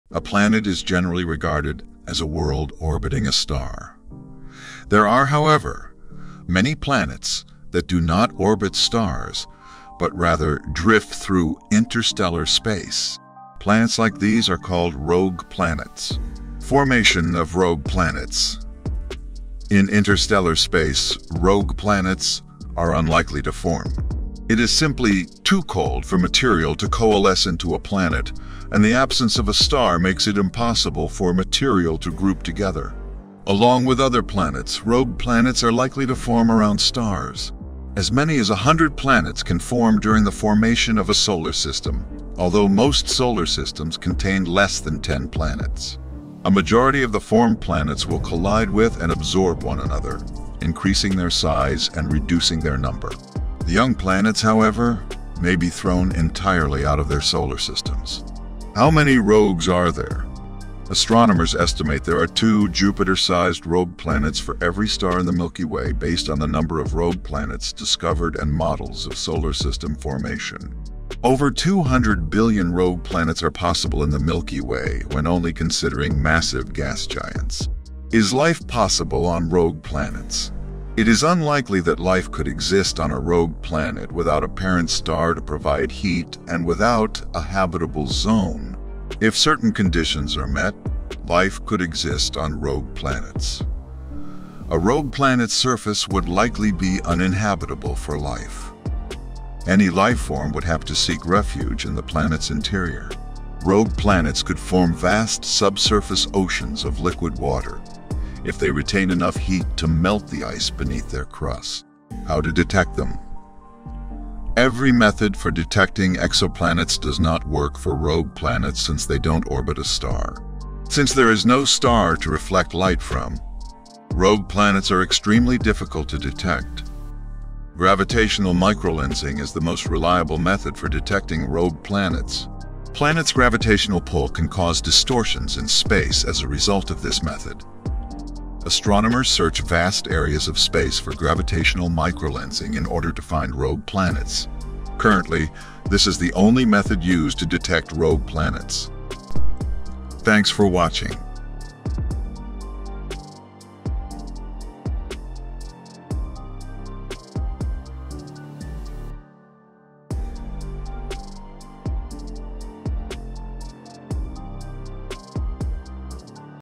What Are Rogue Planets? How to detect them?
A planet is generally regarded as a world orbiting a star. There are, however, many planets that do not orbit stars, but rather drift through interstellar space. Planets like these are called rogue planets.

Formation of rogue planets:

In interstellar space, rogue planets are unlikely to form. It is simply too cold for material to coalesce into a planet, and the absence of a star makes it impossible for material to group together. Along with other planets, rogue planets are likely to form around stars.

 As many as a hundred planets can form during the formation of a solar system, although most solar systems contain less than ten planets. A majority of the formed planets will collide with and absorb one another, increasing their size and reducing their number. The young planets, however, may be thrown entirely out of their solar systems.

How Many Rogues are there? :

Astronomers estimate there are two Jupiter-sized rogue planets for every star in the Milky Way based on the number of rogue planets discovered and models of solar system formation. Over 200 billion rogue planets are possible in the Milky Way when only considering massive gas giants.

Is life possible on rogue planets? :

It is unlikely that life could exist on a rogue planet without a parent star to provide heat and without a habitable zone. If certain conditions are met, life could exist on rogue planets. A rogue planet's surface would likely be uninhabitable for life. Any lifeform would have to seek refuge in the planet's interior. Rogue planets could form vast subsurface oceans of liquid water if they retain enough heat to melt the ice beneath their crust.

How to detect them? :

Every method for detecting exoplanets does not work for rogue planets since they don't orbit a star. Since there is no star to reflect light from, rogue planets are extremely difficult to detect. Gravitational microlensing is the most reliable method for detecting rogue planets. Planets' gravitational pull can cause distortions in space as a result of this method.

 Astronomers search vast areas of space for gravitational microlensing in order to find rogue planets. Currently, this is the only method used to detect rogue planets.